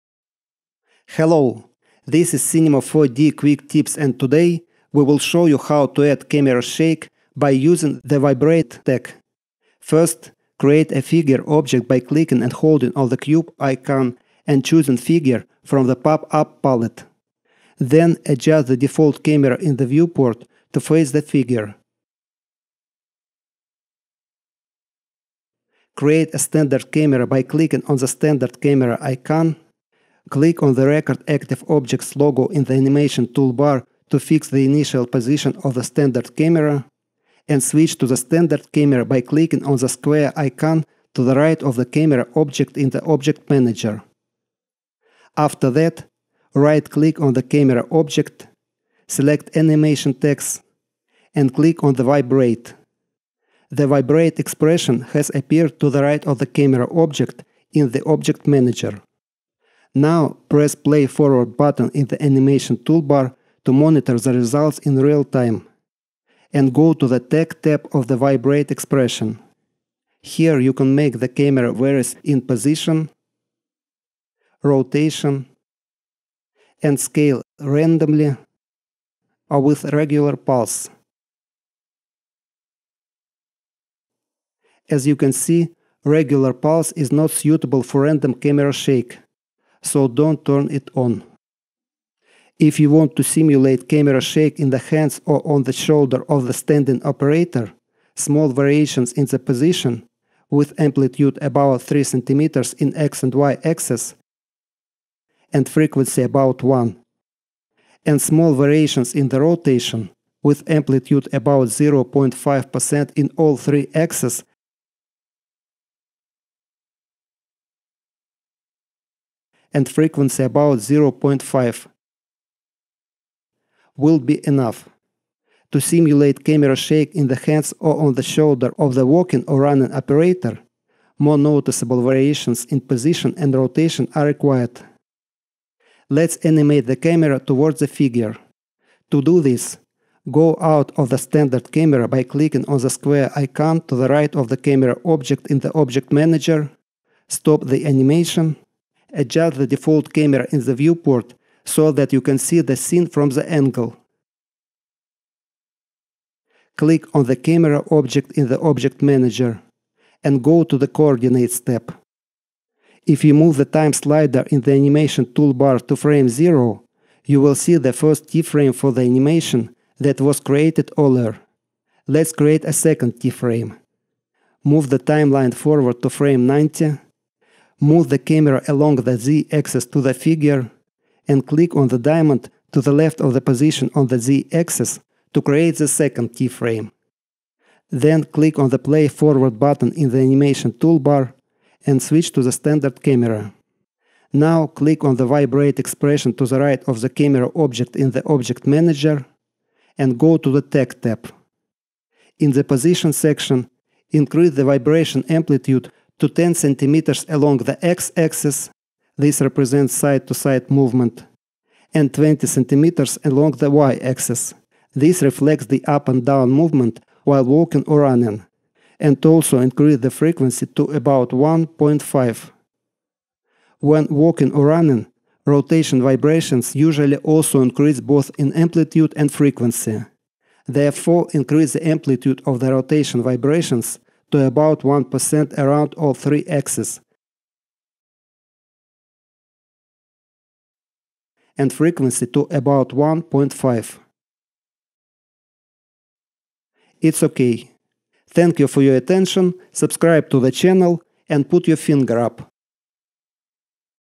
Cinema 4D: Camera Shake using the Vibrate tag
How to add Camera Shake using the Vibrate tag in Cinema 4D 2024.